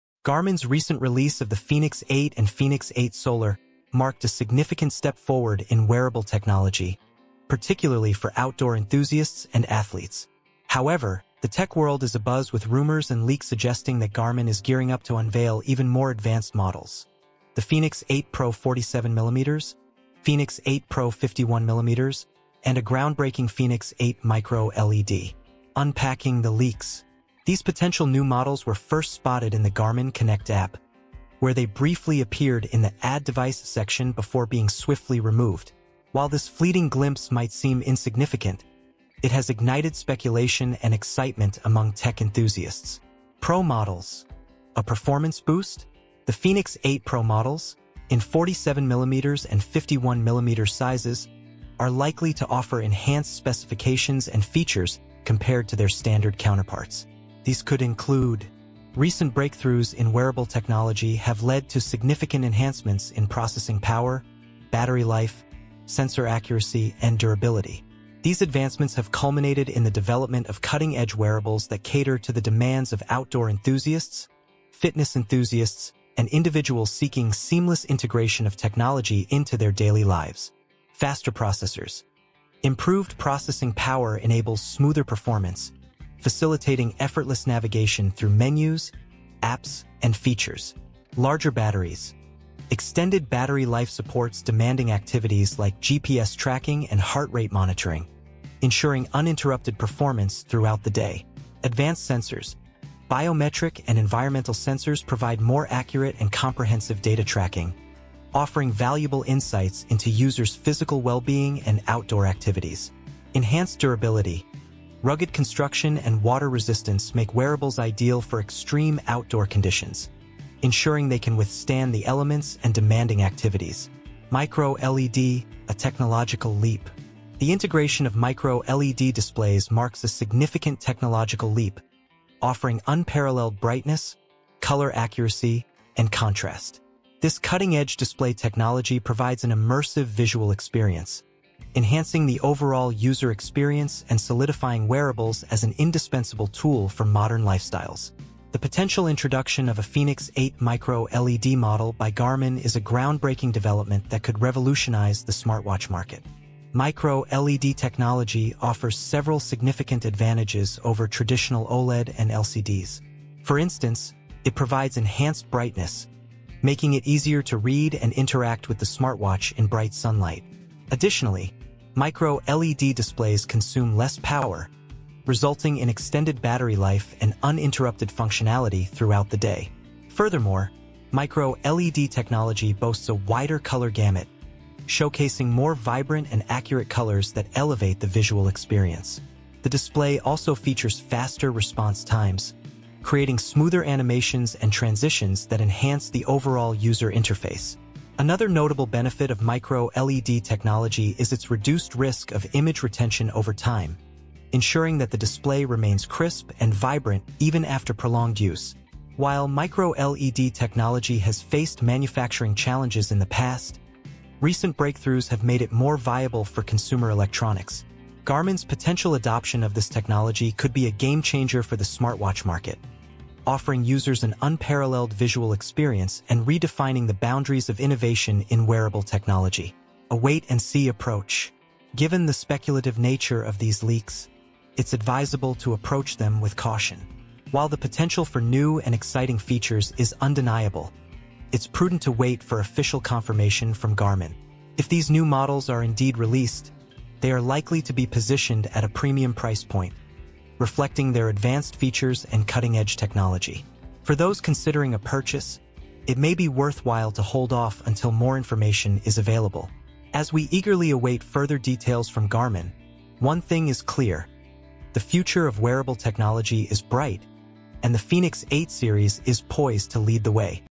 Garmin Fenix 8 Pro and MicroLED - First Official Leak!
Garmin's recent release of the Phoenix 8 and Phoenix 8 Solar marked a significant step forward in wearable technology, particularly for outdoor enthusiasts and athletes. However, the tech world is abuzz with rumors and leaks suggesting that Garmin is gearing up to unveil even more advanced models: the Phoenix 8 Pro 47mm, Phoenix 8 Pro 51mm, and a groundbreaking Phoenix 8 micro LED. These potential new models were first spotted in the Garmin Connect app, where they briefly appeared in the "add device" section before being swiftly removed. While this fleeting glimpse might seem insignificant, it has ignited speculation and excitement among tech enthusiasts.